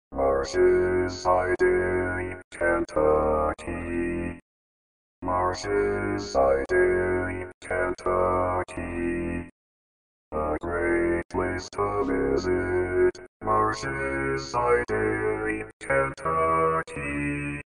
How to Say or Pronounce USA Cities — Marshes Siding, Kentucky
This video shows you how to say or pronounce Marshes Siding, Kentucky.

A computer said Marshes Siding, Kentucky. How would you say Marshes Siding, Kentucky?